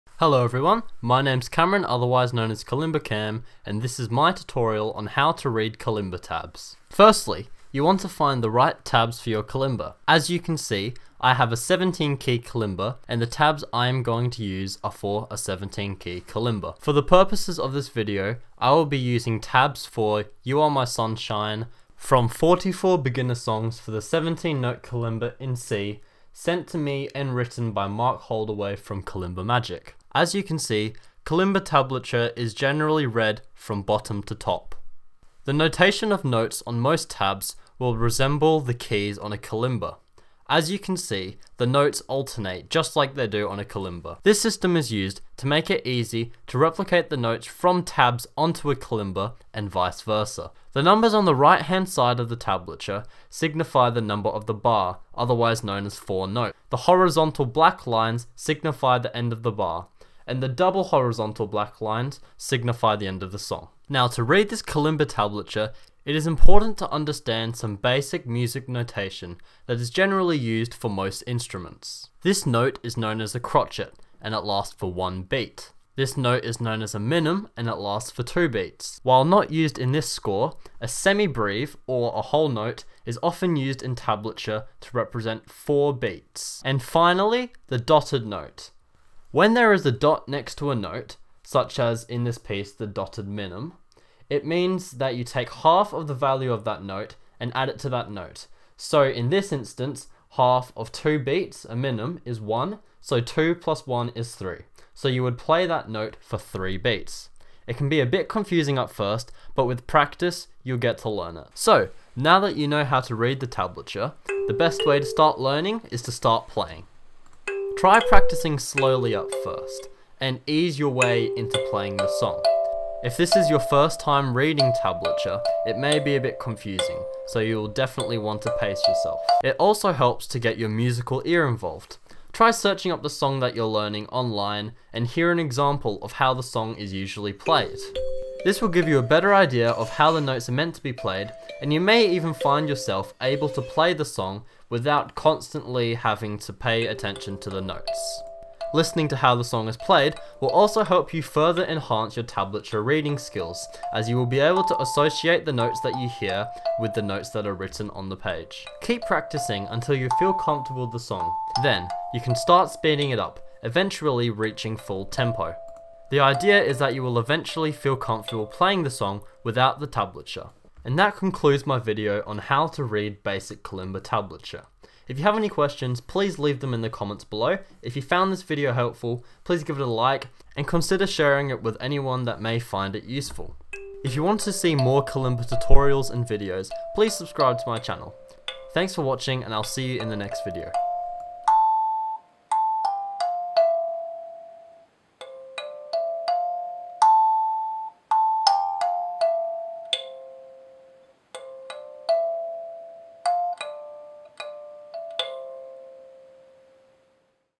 How to Read Kalimba Tablature (Tabs)
Hello everyone! In this video, I explain how to read basic Kalimba tablature (tabs). In this particular video,  I use the 17-key Kalimba tuned to C Major from Muspor. I cover using the correct kind of tablature, how tablature is structured/written, musical notation, and how to practice with the tabs.

Links:
🎙️ MICROPHONE USED:
Blue Snowball USB Mic (Black)

📷CAMERA USED:
Nikon D7000